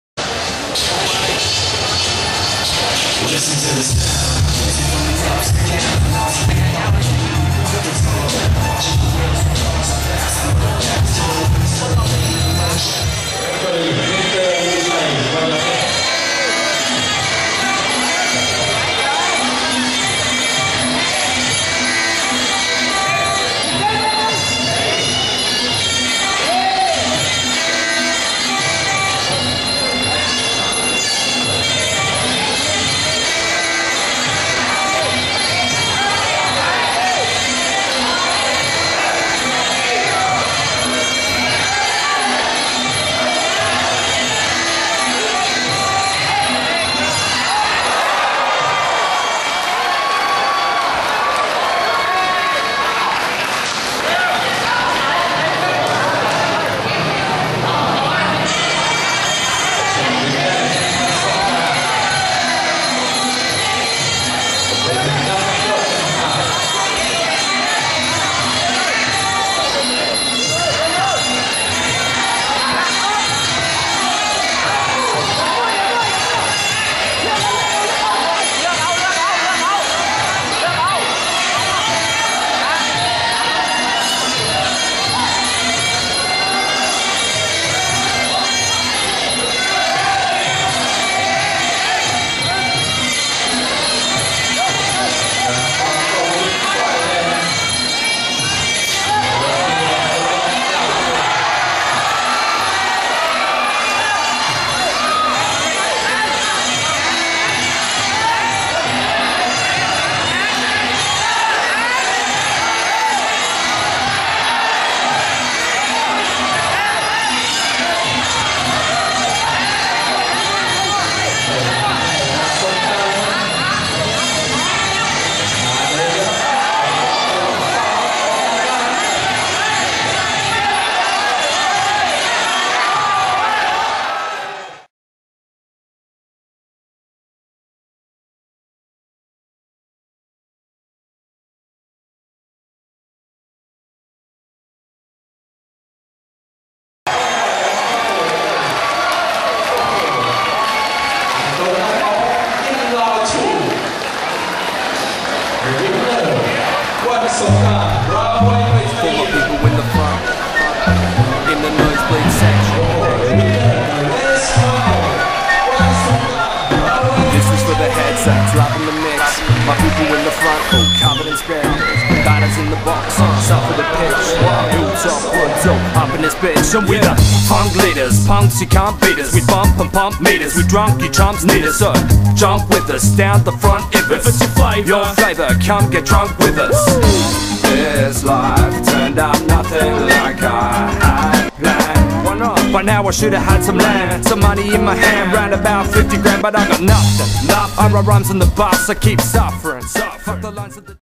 Rawai Muay Thai trainer Songkran wins at Bangla: 8 September 2010
Songkran is trainer at Rawai Muay Thai camp in Phuket, Thailand. He was a popular boxer in Bangkok, back in the days. Sorry, Round 1 is missing... I accidentally erased it.